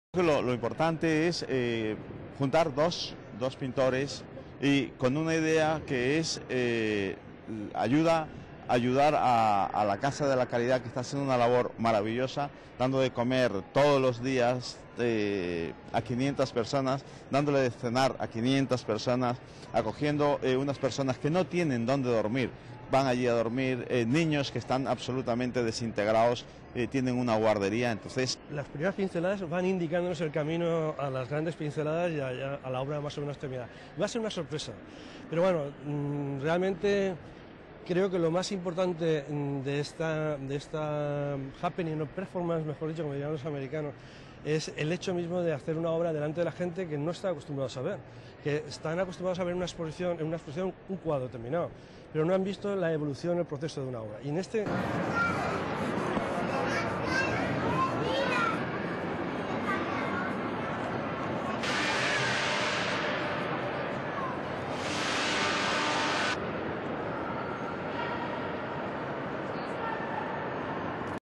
Ramos y Silva donan cuadro a Casa de la Caridad
Los artistas Willy Ramos y Horacio Silva han pintado  a cuatro manos un cuadro en el Museo de las Ciencias Príncipe Felipe de Valencia, cuyos visitantes han podido ver todo el proceso de elaboración. La obra será donada a la Casa de la Caridad de Valencia.